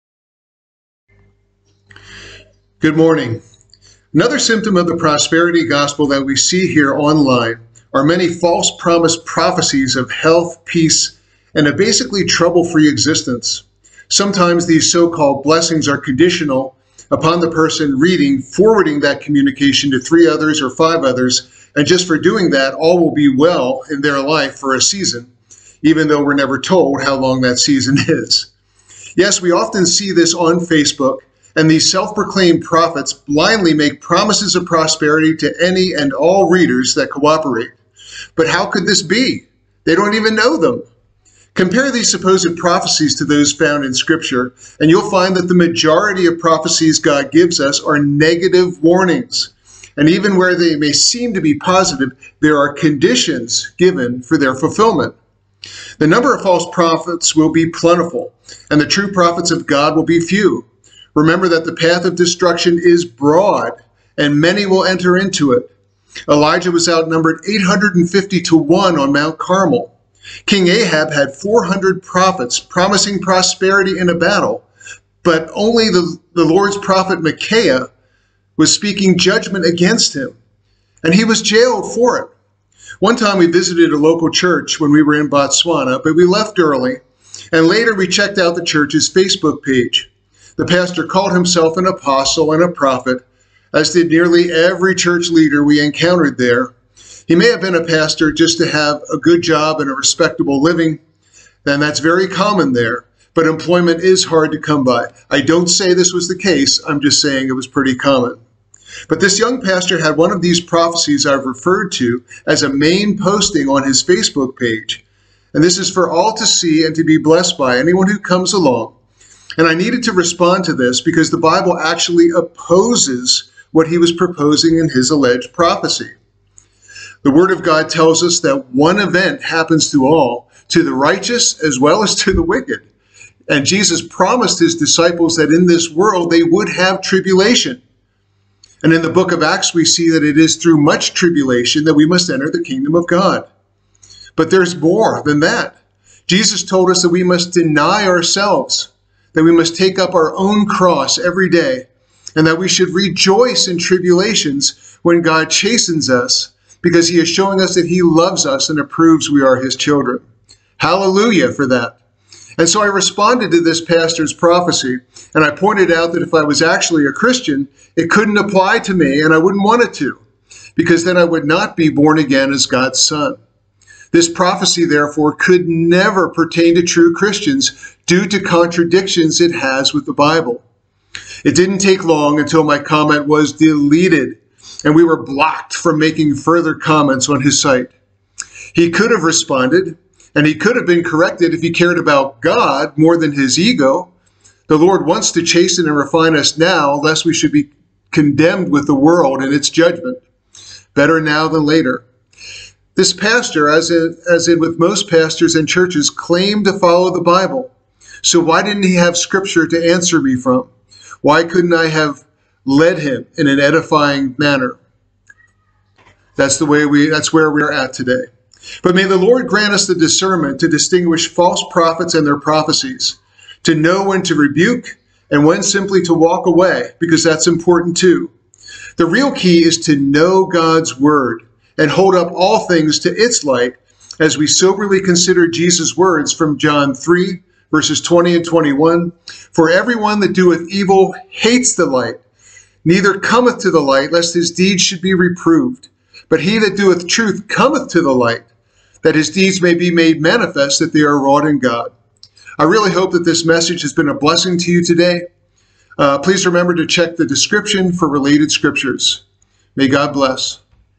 When prophecy isn't valid
Here are some scriptures related to this video:

Discerning Spirits:
1 Kings chapters 18 & 22
Proverbs 26:4,5
Luke 6:26
Hebrews 5:14
John 3:20,21

Believers' Tribulations:
Ecclesiastes 9:2
Matthew 5:11,12; 7:13,14
Luke 9:23
John 16:33
Acts 14:22
Romans 5:3-5
1 Corinthians 11:31,32
1 Peter 1:6,7; 5:8-10
Revelation 3:19